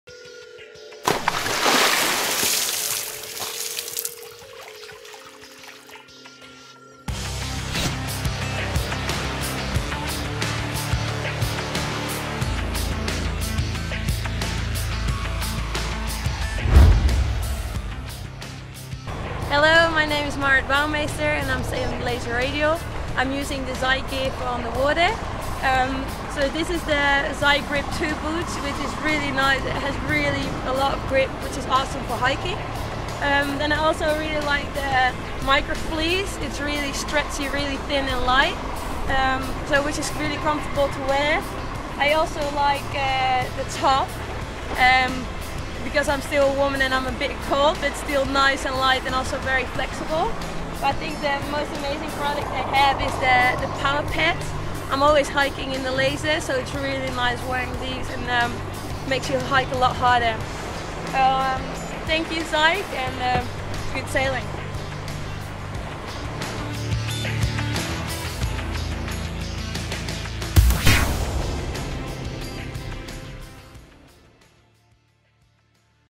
ZHIK TV - What's in your kit bag? - Marit Bouwmeester
One of the worlds best laser radial sailors, Marit Bouwmeester, talks to us about the gear she always has in her kit bag.